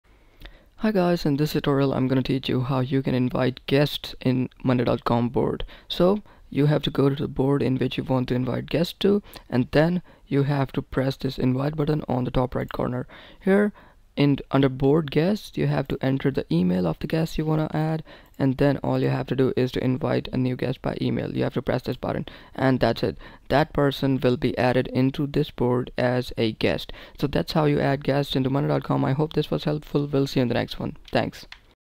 How to Invite Guests in Monday Com [Quick Guide]
In this tutorial, you will learn how to invite guest in monday com. It is an easy learning curve to learn how to add guest in monday.com, and in this tutorial we will show you everything you need to know.